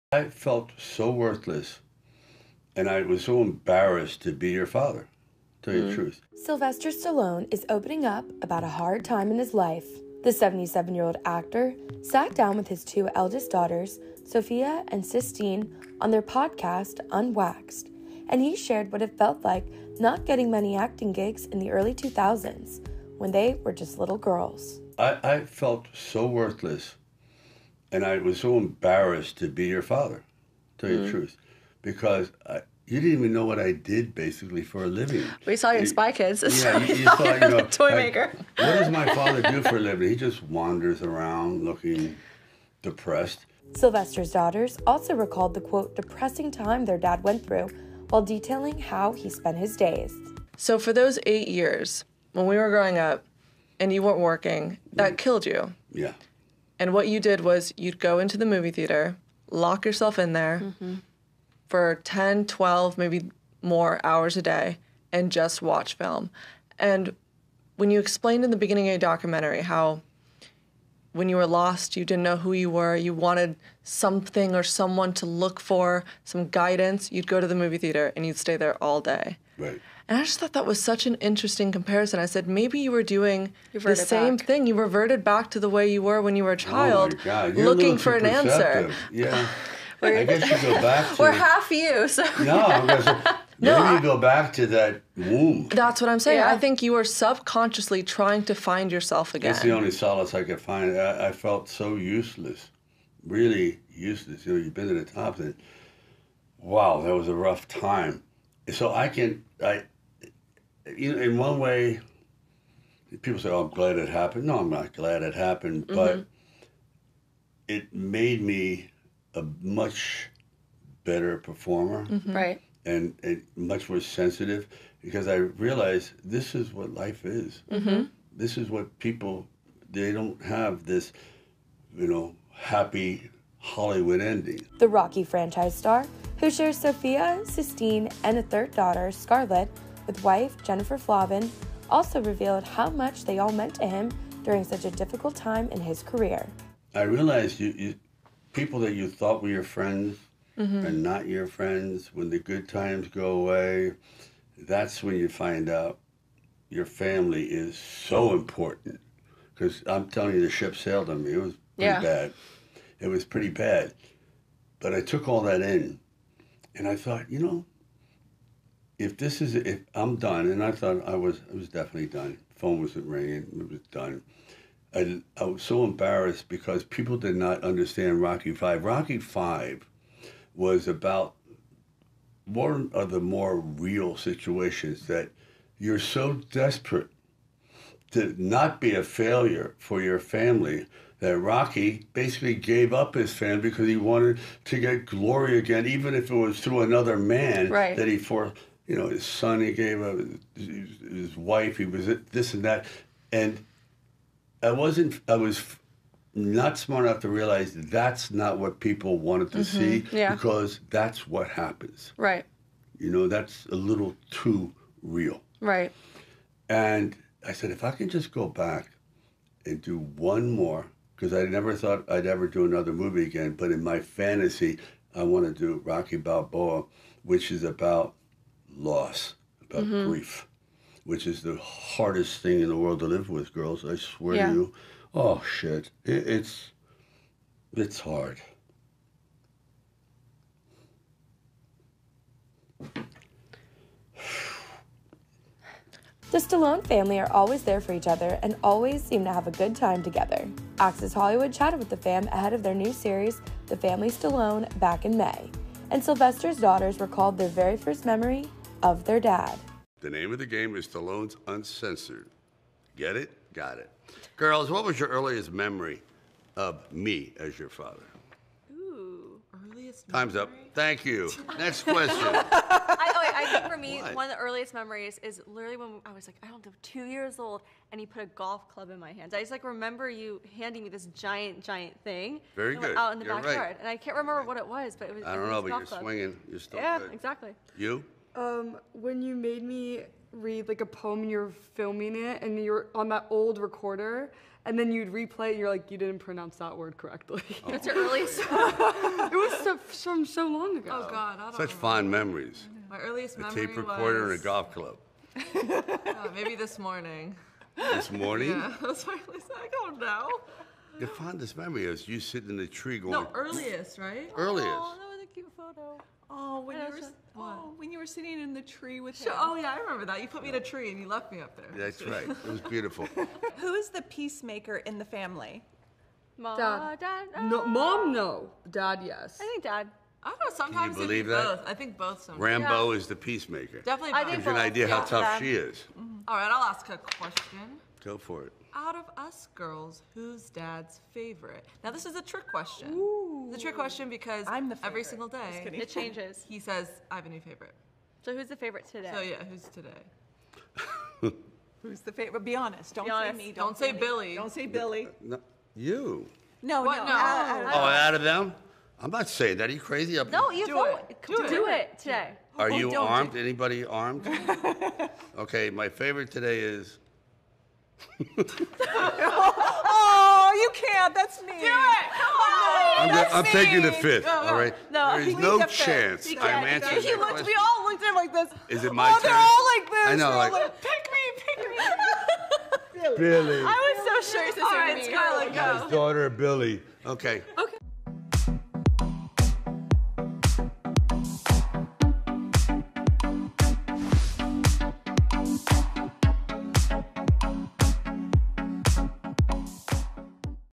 Sylvester Stallone Opens Up To His Daughters About Difficult Time In His Life
Sylvester Stallone is opening up about a hard time in his life. The 77-year-old actor sat down with his two eldest daughters, Sophia and Sistine, on their podcast "Unwaxed," and he shared what it felt like not getting any acting gigs in the early 2000s when they were little girls. "I was so embarrassed to be your father," he said. Sylvester's daughters also recalled the "depressing" time their dad went through while detailing how he watched movies for "10 to 12 hours every day." The "Rocky" franchise star, who shares Sophia, Sistine and a third daughter Scarlet with wife Jennifer Flavin, also revealed how much they all meant to him during such a difficult time in his career. "When the good times go away, that’s when you find out your family is so important. The ship sailed on me; it was pretty bad. And I realized that people you thought were your friends are not your friends. I thought I was done. The phone wasn't ringing," he said to his daughters.